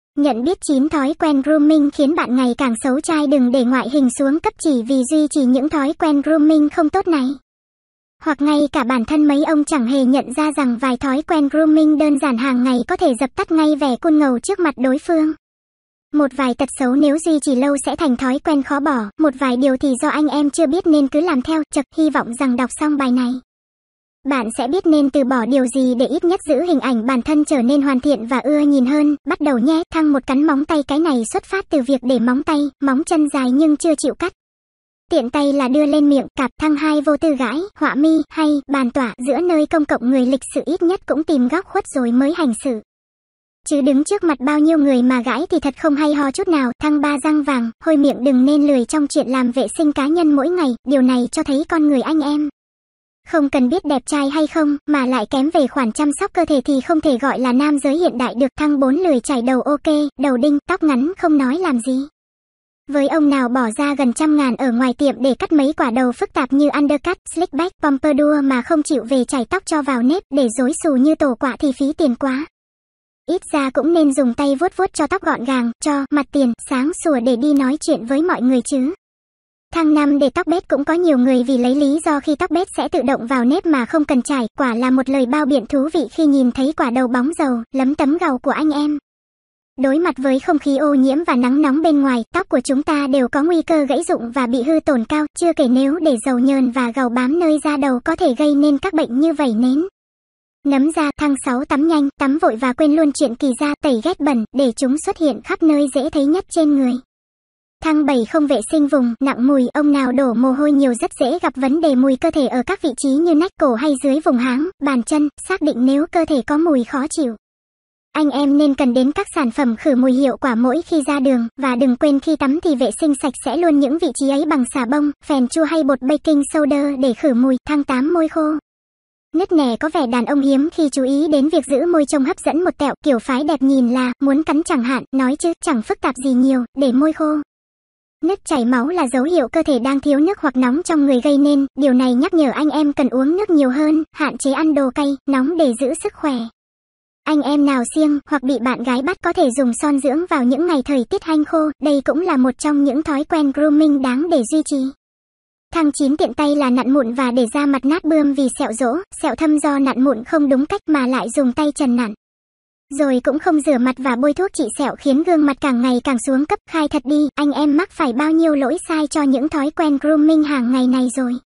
Nhận biết 9 thói quen grooming khiến bạn ngày càng xấu trai
Đừng để ngoại hình xuống cấp chỉ vì duy trì những thói quen grooming không tốt này.

Hoặc ngay cả bản thân mấy ông chẳng hề nhận ra rằng vài thói quen grooming đơn giản hàng ngày có thể dập tắt ngay vẻ cool ngầu trước mặt đối phương.

Một vài tật xấu nếu duy trì lâu sẽ thành thói quen khó bỏ, một vài điều thì do anh em chưa biết nên cứ làm theo.

Chậc, hy vọng rằng đọc xong bài này, bạn sẽ biết nên từ bỏ điều gì để ít nhất giữ hình ảnh bản thân trở nên hoàn thiện và ưa nhìn hơn.

Bắt đầu nhé.

#1 Cắn móng tay

Cái này xuất phát từ việc để móng tay, móng chân dài nhưng chưa chịu cắt, tiện tay là đưa lên miệng.. cạp.

#2 Vô tư gãi "họa mi" hay "bàn tọa" giữa nơi công cộng

Người lịch sự ít nhất cũng tìm góc khuất rồi mới hành sự, chứ đứng trước mặt bao nhiêu người mà gãi thì thật không hay ho chút nào.

#3 Răng vàng/Hôi miệng

Đừng nên lười trong chuyện làm vệ sinh cá nhân mỗi ngày.

Điều này cho thấy con người anh em, không cần biết đẹp trai hay không, mà lại kém về khoản chăm sóc cơ thể thì không thể gọi là nam giới hiện đại được.

#4 Lười chải đầu

Ok, đầu đinh, tóc ngắn không nói làm gì.

Với ông nào bỏ ra gần trăm ngàn ở ngoài tiệm để cắt mấy quả đầu phức tạp như Undercut, Slicked-back, Pompadour mà không chịu về chải tóc cho vào nếp, để rối xù như tổ quạ thì phí tiền quá.

Ít ra cũng nên dùng tay vuốt vuốt cho tóc gọn gàng, cho "mặt tiền" sáng sủa để đi nói chuyện với mọi người chứ.

#5 Để tóc bết

Cũng có nhiều người vì lấy lý do khi tóc bết sẽ tự động vào nếp mà không cần chải.

Quả là một lời bao biện thú vị khi nhìn thấy quả đầu bóng dầu, lấm tấm gàu của anh em.

Đối mặt với không khí ô nhiễm và nắng nóng bên ngoài, tóc của chúng ta đều có nguy cơ gãy rụng và bị hư tổn cao, chưa kể nếu để dầu nhờn và gàu bám nơi da đầu có thể gây nên các bệnh như vẩy nến, nấm da,...

#6 Tắm nhanh, tắm vội

Và quên luôn chuyện kỳ da, tẩy ghét bẩn, để chúng xuất hiện khắp nơi dễ thấy nhất trên người.

#7 Không vệ sinh vùng "nặng mùi"

Ông nào đổ mồ hôi nhiều rất dễ gặp vấn đề mùi cơ thể ở các vị trí như nách, cổ hay dưới vùng háng, bàn chân,...

Xác định nếu cơ thể có mùi khó chịu, anh em nên cần đến các sản phẩm khử mùi hiệu quả mỗi khi ra đường.

Và đừng quên khi tắm thì vệ sinh sạch sẽ luôn những vị trí ấy bằng xà bông, phèn chua hay bột baking soda để khử mùi.

#8 Môi khô, nứt nẻ

Có vẻ đàn ông hiếm khi chú ý đến việc giữ môi trông hấp dẫn một tẹo, kiểu phái đẹp nhìn là... muốn cắn chẳng hạn?

Nói chứ, chẳng phức tạp gì nhiều, để môi khô, nứt chảy máu là dấu hiệu cơ thể đang thiếu nước hoặc nóng trong người gây nên.

Điều này nhắc nhở anh em cần uống nước nhiều hơn, hạn chế ăn đồ cay, nóng để giữ sức khỏe.

Anh em nào siêng (hoặc bị bạn gái bắt) có thể dùng son dưỡng vào những ngày thời tiết hanh khô, đây cũng là một trong những thói quen grooming đáng để duy trì.

#9 Tiện tay là nặn mụn

Và để da mặt nát bươm vì sẹo rỗ, sẹo thâm do nặn mụn không đúng cách.

Mà lại dùng tay trần nặn, rồi cũng không rửa mặt và bôi thuốc trị sẹo khiến gương mặt càng ngày càng xuống cấp.

Khai thật đi, anh em mắc phải bao nhiêu lỗi sai cho những thói quen grooming hàng ngày này rồi?